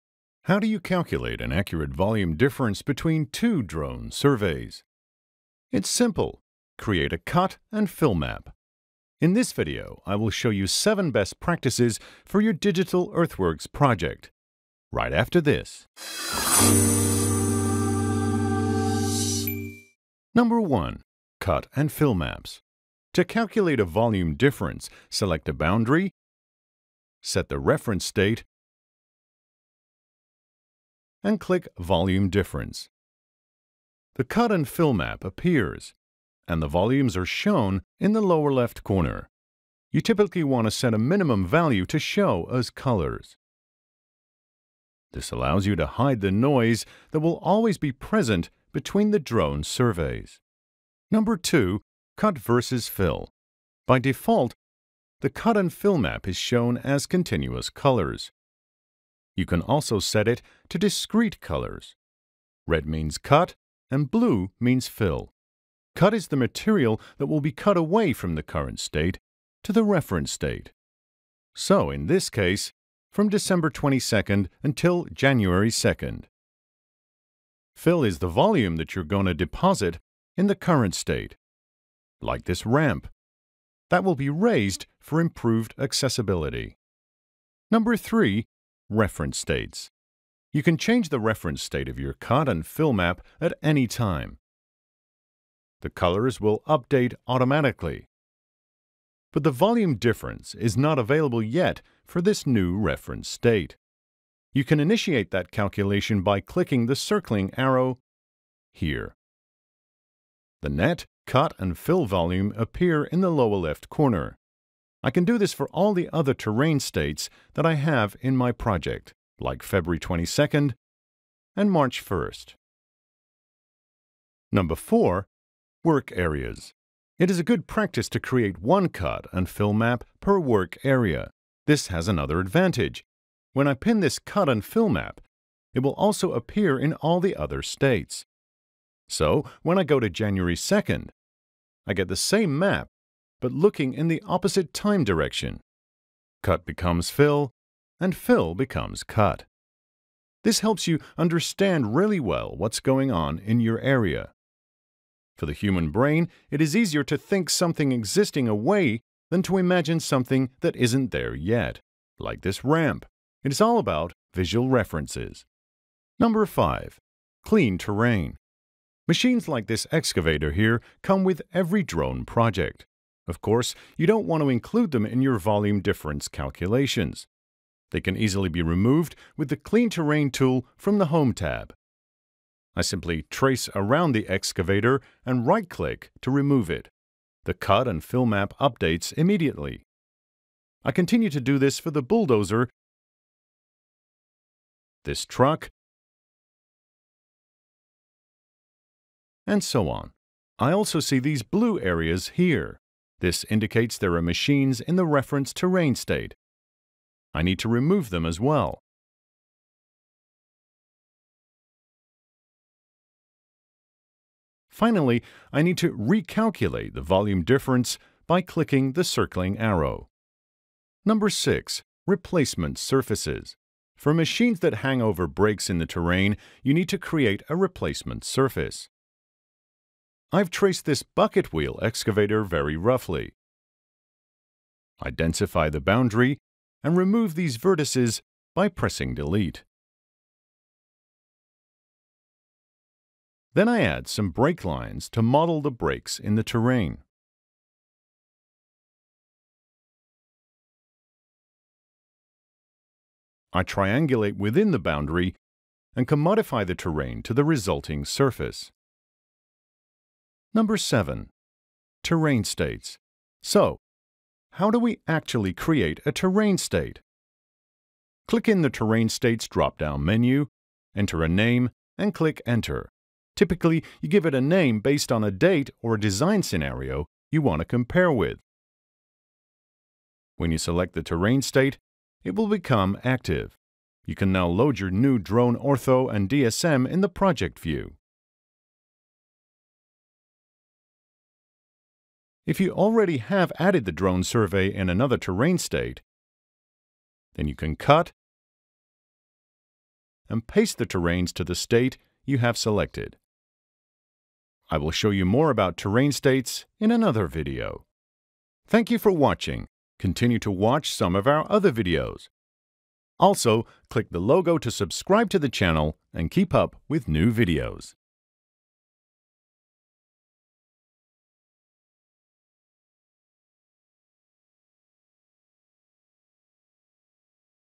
7 Best Practices for Cut/Fill Maps
You can create a volume difference between 2 drone surveys with a cut/fill map. This video explains 7 best practices when working with cut/fill maps.

1) Cut/Fill Maps

You can create a cut/fill map by selecting a boundary, setting the proper reference and clicking the Volume Difference button. The cut/fill map is displayed in the viewport and the volume difference calculation appears in the lower left corner. You typically want to set a minimum value to show as colors. This allows you to hide the noise that will always be present between the drone surveys.

2) Cut versus Fill

By default, the cut/fill map is shown as continuous colors. You can also set it to discrete colors. Red means cut and blue means fill. Cut is the material that will be cut away from the current state to the reference state. Fill is the volume that is going to be deposited in the current state like a ramp that is built for improved accessibility.

3) Reference states

You can change the reference terrain state of your cut/fill map at any time. The colors will update automatically. The volume difference might not be available yet for this set reference state. You can initiate that calculation by clicking the circling arrow. The net, cut and fill volume appear in the lower left corner.

4) Work areas

It is a good practice to create one cut/fill map per work area. This has the advantage that when you pin the cut/fill map, it will also appear in all the other states. This will help you to monitor changes over time. Also, it allows to switch quickly between your current terrain state and the reference state. Cut becomes fill and fill becomes cut. This helps you understand really well what’s going on in your area. For the human brain it is easier to think something existing away then to imagine something that isn’t there yet.

5) Clean Terrain

Machines like excavators come with every earthworks project. You don’t want to include them in your volume difference calculations. They can be removed easy with the Clean Terrain tool from the Home tab. You simply trace around the machine and right click to remove it. The cut/fill map updates immediately.  Also, unexpected colors indicate there might be machines in the reference state. You need to remove those as well in order to get an accurate volume difference measurement. When you remove an object, you can recalculate the volume difference by clicking the circling arrow.

6) Replacement surfaces

For machines that hang over breaks in the terrain you need to create a replacement surface. Trace around the machine and densify the boundary. Remove the vertices that are on the machine by pressing delete. Then add some breaklines to model the breaks in the terrain. Triangulate within the boundary and modify the terrain to the resulting replacement surface.

7) Terrain States

You can create a terrain state by clicking in the dropdown menu above the project view. Enter a name and click Enter. Typically, enter a date or the name of the design scenario as the name for the terrain state. When you select the state, it will become active and you can start adding your orthophoto and DSM. You can also Cut (Ctrl+X) and Paste (Ctrl+V) orthophotos and DSMs from one state to another. Terrain states are described in detail in a dedicated movie.

Virtual Surveyor is smart drone surveying software.